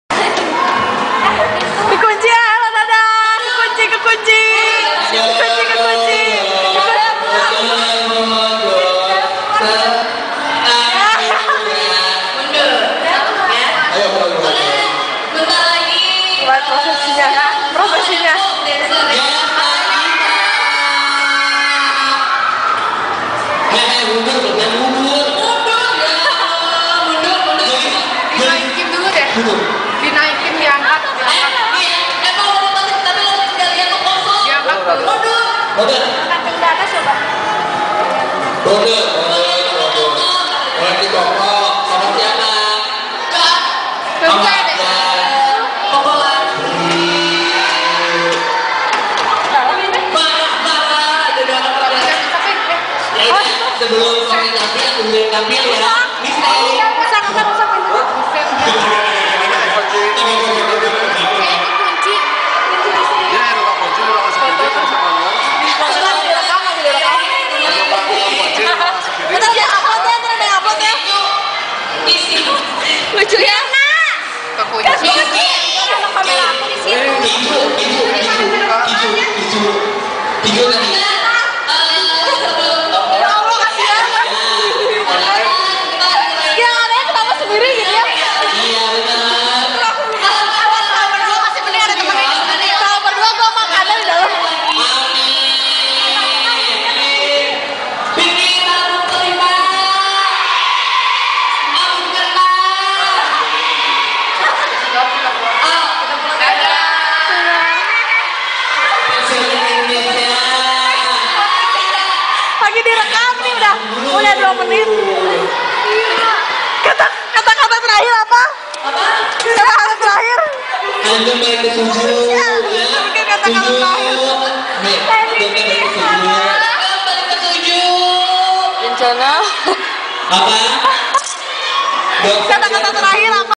SHiny Girls's Taemin was locked!!
This incident occurred after our performances at the gathering elf.

sorry for the bad camera angle~ :(

poor taeminnie..